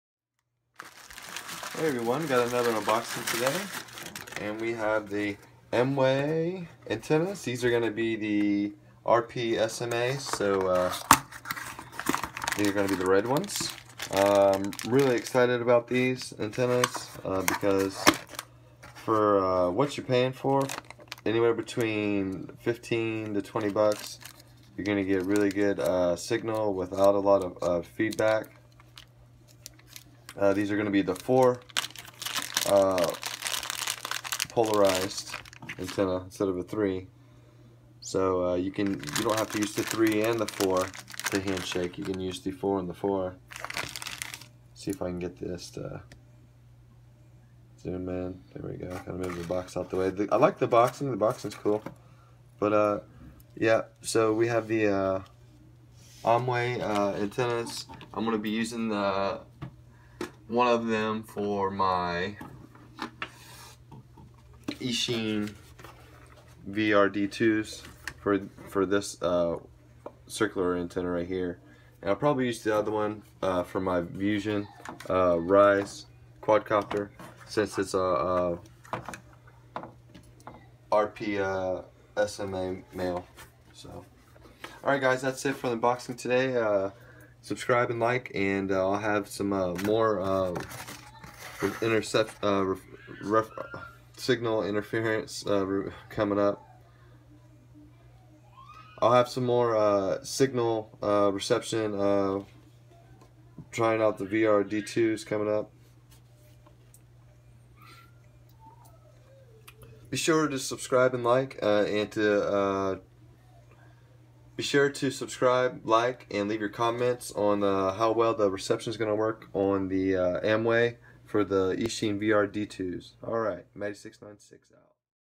UnBoxing - AOMWAY L-RP SMA 5.8ghz FPV Antenna RX/TX best value?
A Simple UnBoxing:AOMWAY RP-SMA FPV Antenna RX/TX. Could it be the best bang for you budget?